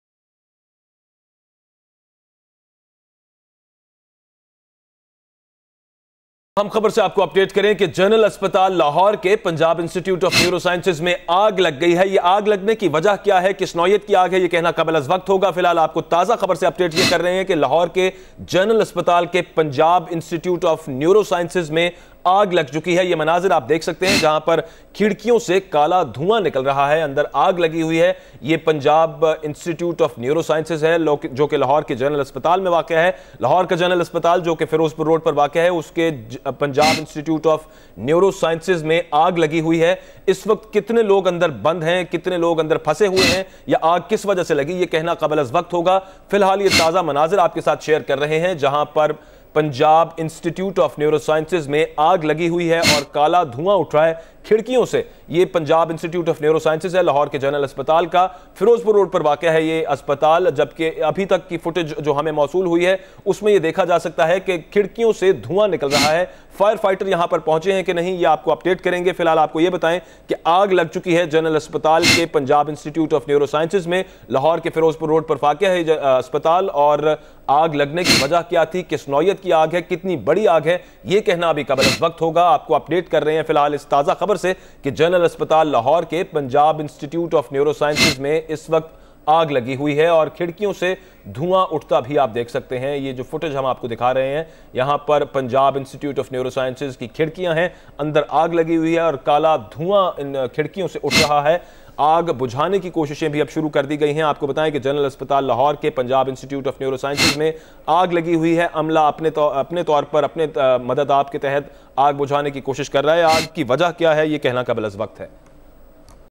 Punjab Institute of Neurosciences in General Hospital Catches Fire | 24 News HD
24 News HD is one of the leading news channels of Pakistan bringing you the latest current affairs from Pakistan and around the world.